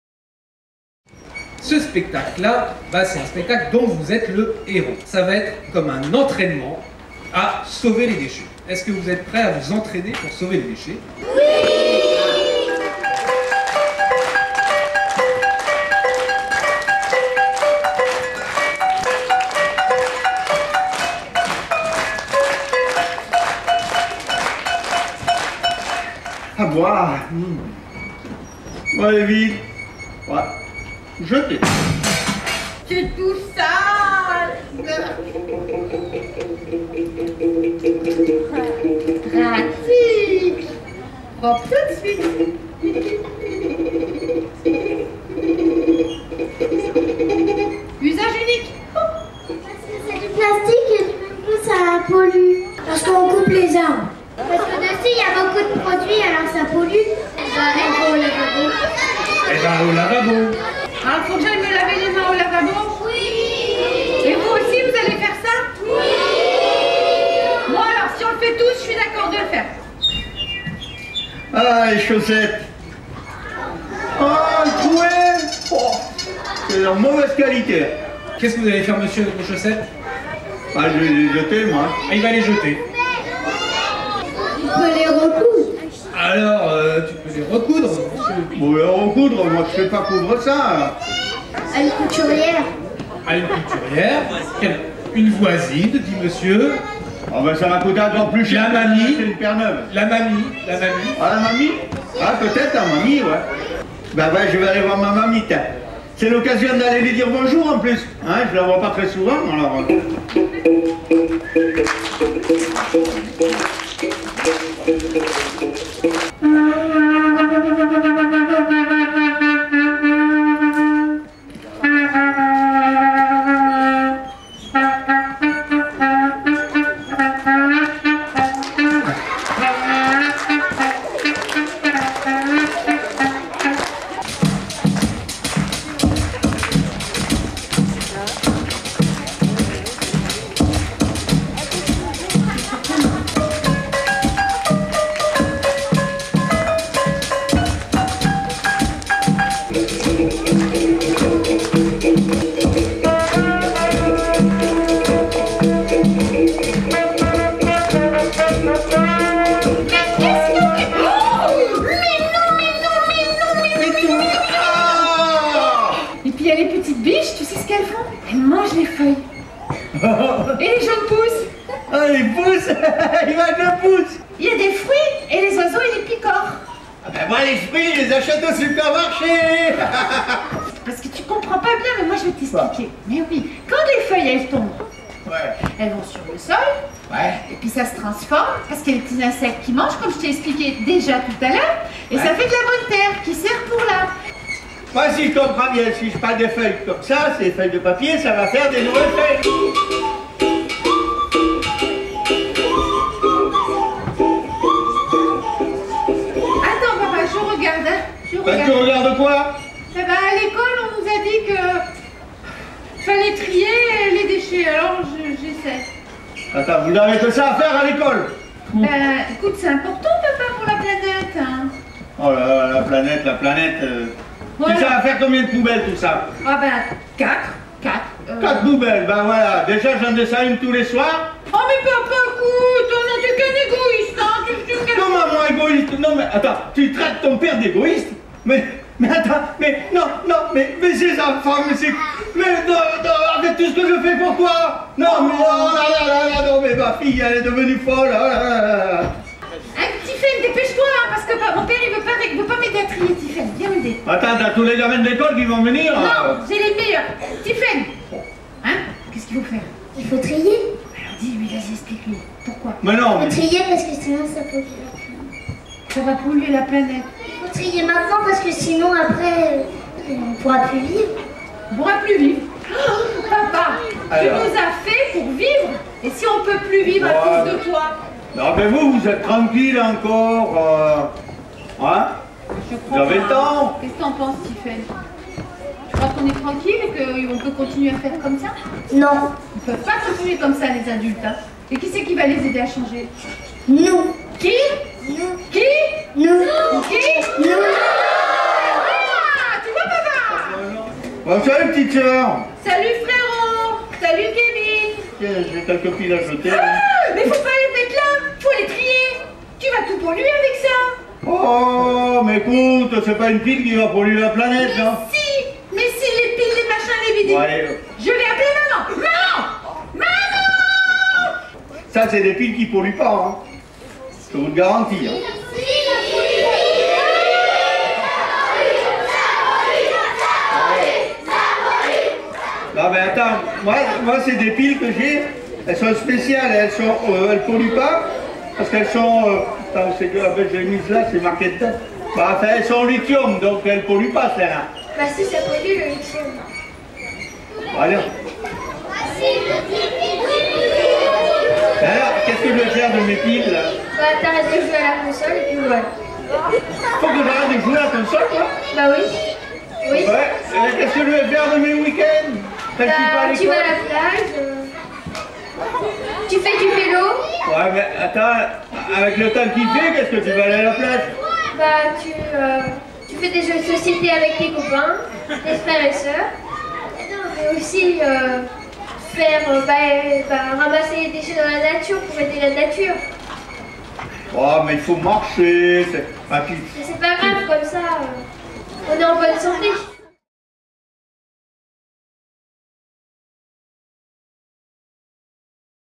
Spectacle participatif pour enfant sur les déchet : "ça sert encore"
Théâtre participatif pour enfants, 6--12 ans, sur les problématiques des déchets et les solutions : tri sélectif, valorisation, réparation, réemploi, compostage, réduction à la source...

Co-prodution éCohérence et Cie des Echomédiens.

Vidéo de la représentation du 21/11/2012 Au Havre lors de la Semaine Européenne de réduction des déchets pour la CODAH.